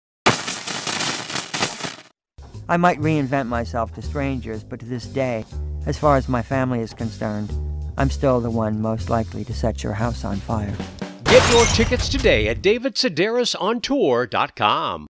Straz Center - David Sedaris | Tampa
Following the release of his newest book Happy Go Lucky, David Sedaris will be live on stage in Tampa for one night only. Sedaris is the author of the bestsellers Calypso, Naked, Me Talk Pretty One Day, Dress Your Family in Corduroy and Denim and a regular National Public Radio contributor.